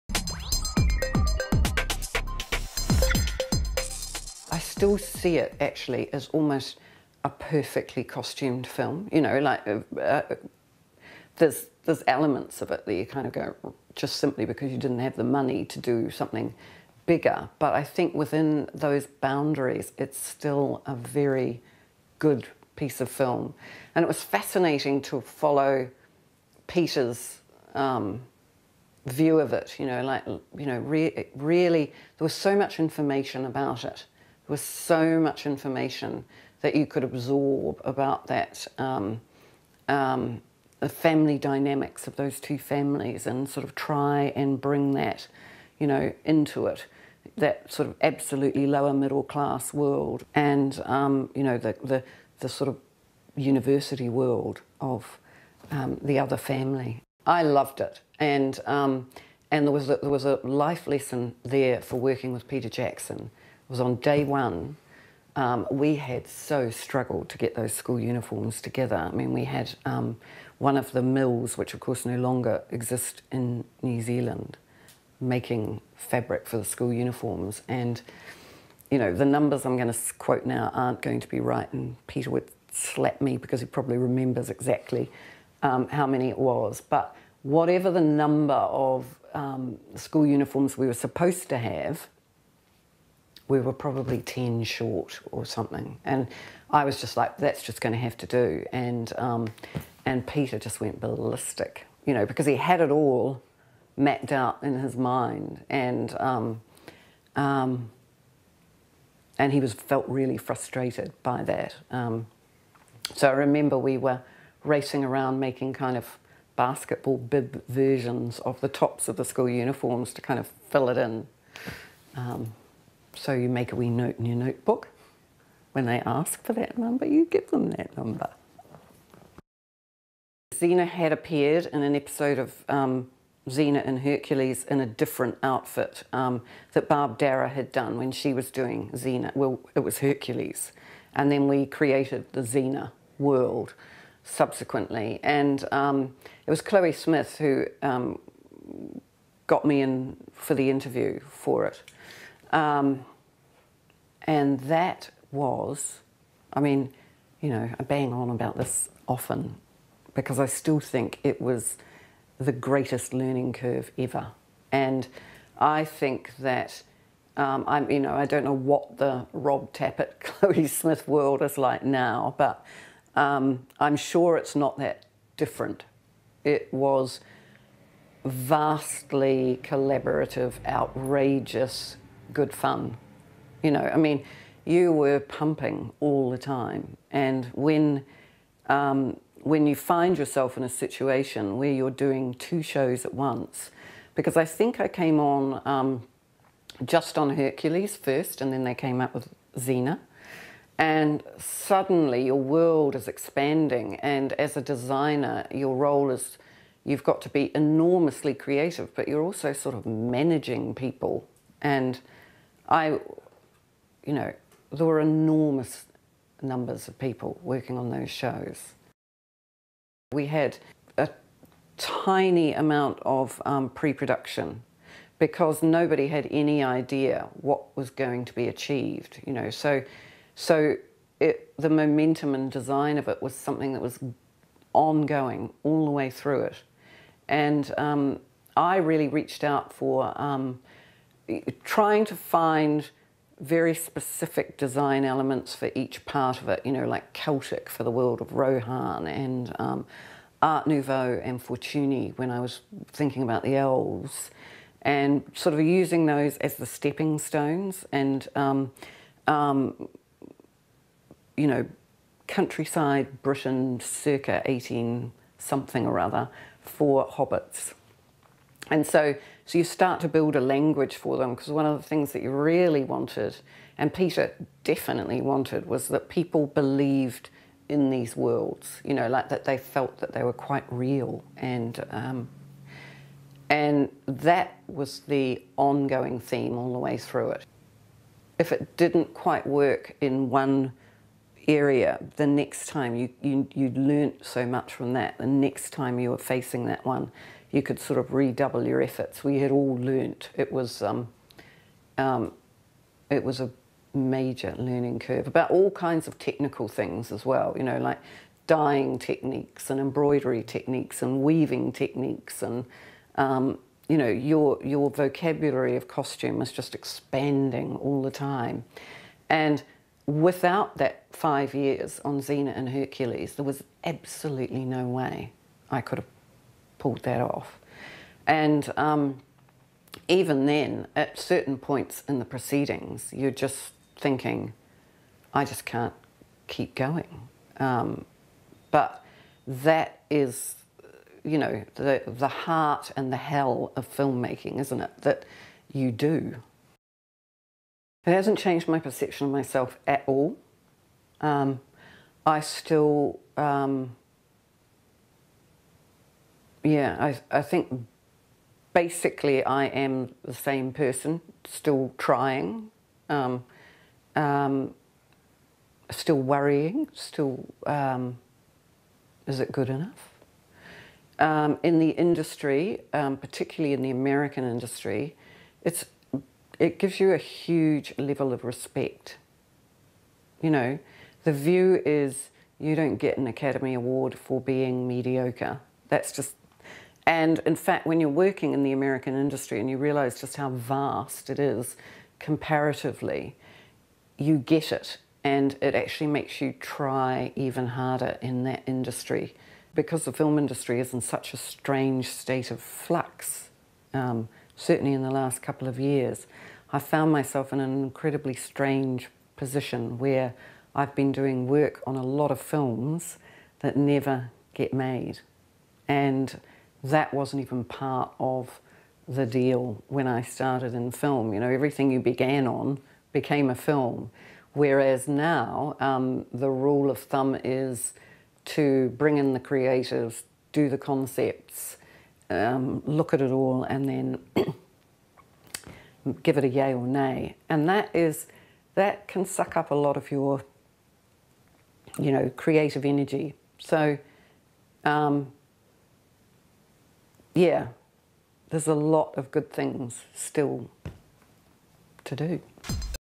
Ngila Dickson Interview
Interview with Ngila Dickson costume designer, in it she talks about Heavenly Creatures, Xena, Lord of The Rings.

this interview was conducted by screentalk.nzonscreen.com.

Date: 2013